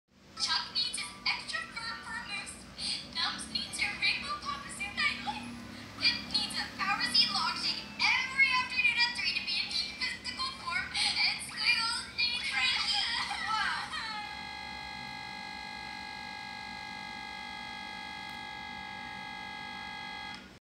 Frankie Pamplemousse Crying Has BSOD
And Squiggles Needs... (Cries) [BSOD]

Name: IRQL_GT_ZERO_AT_SYSTEM_SERVICE

STOP: 0X0000004A (0X81E6FE9C , 0X00000002 , 0X00000000 , 0X00000000)

myfault.sys: Address 81E6FE9C base at 81E6F000 DateStamp 5d014729